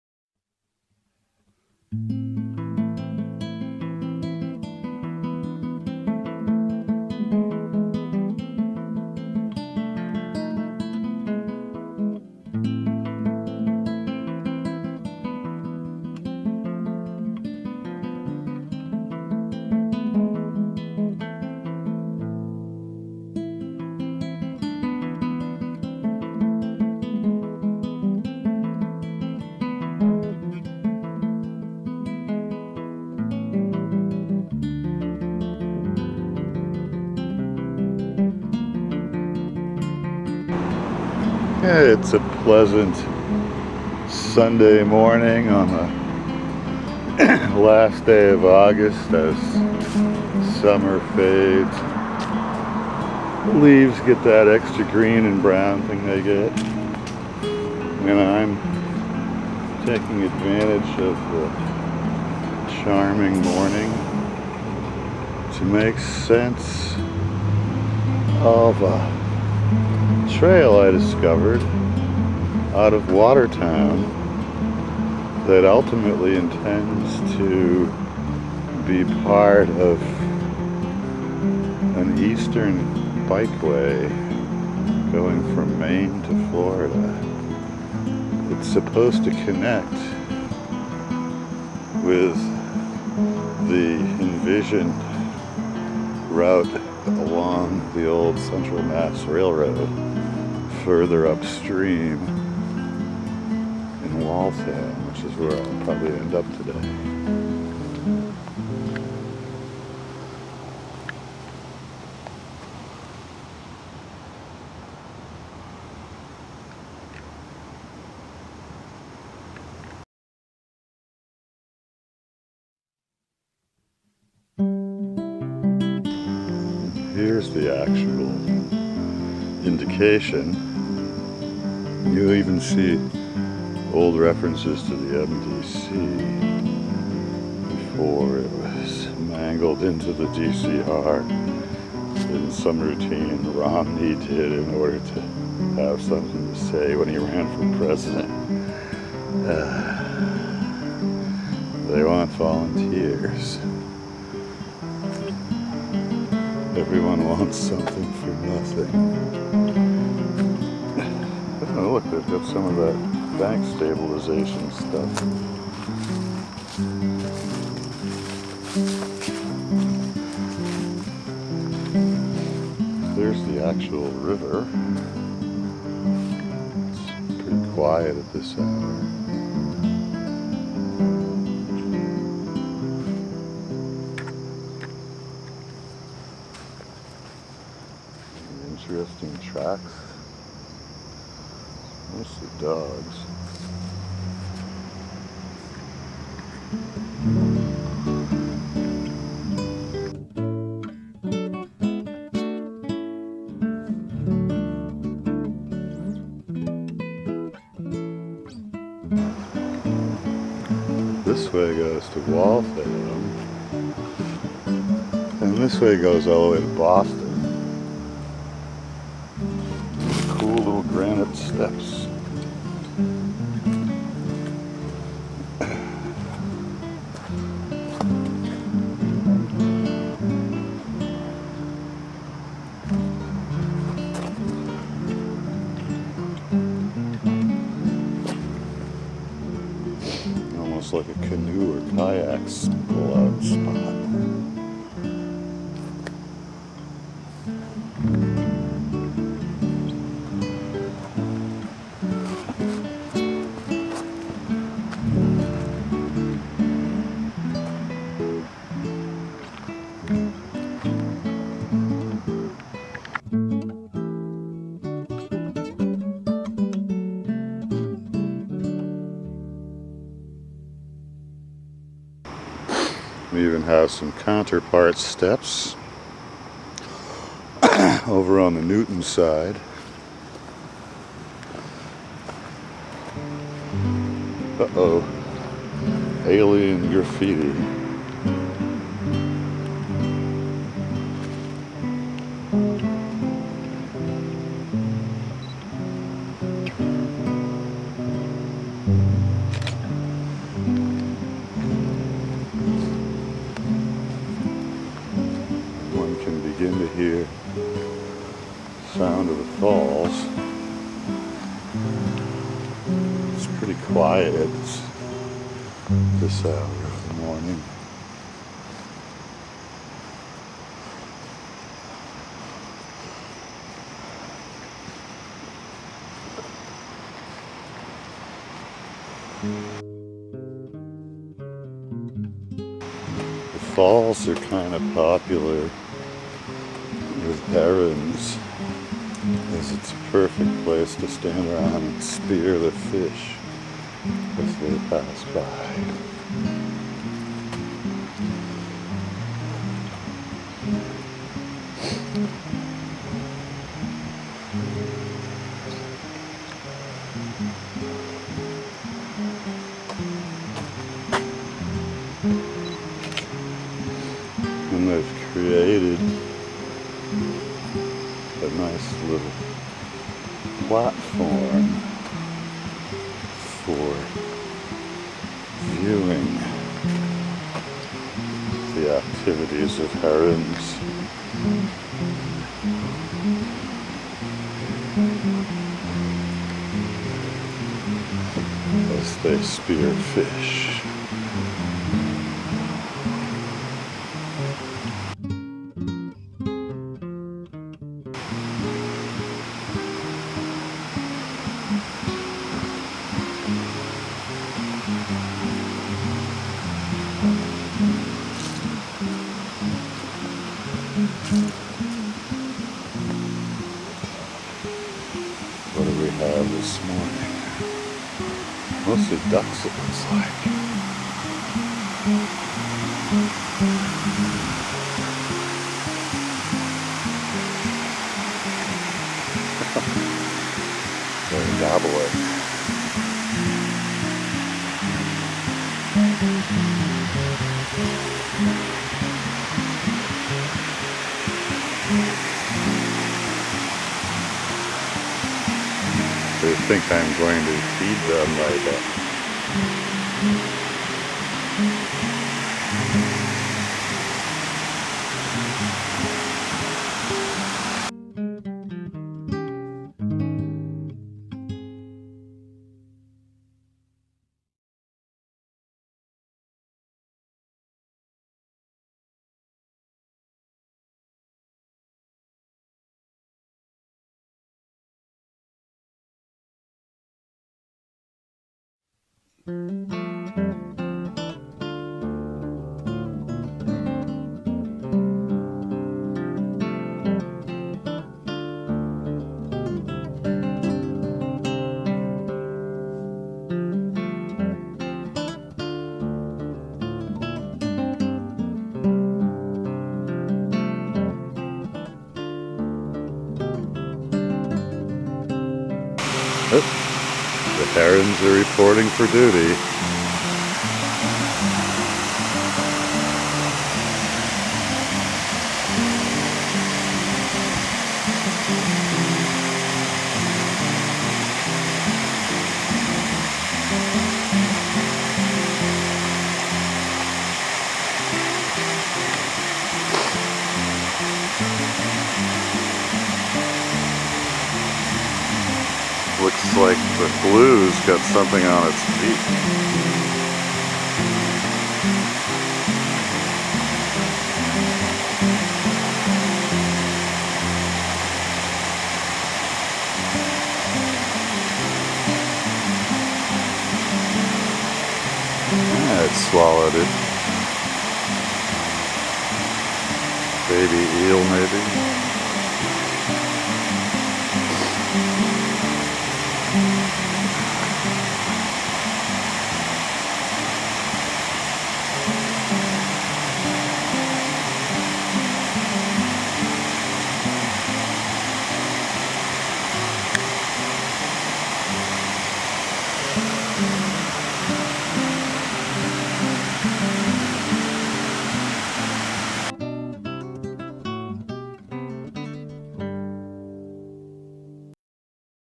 Charles River Greenways Watertown MA Part 1.
From Watertown Square, upstream, the Charles is lined with Greenways on both banks.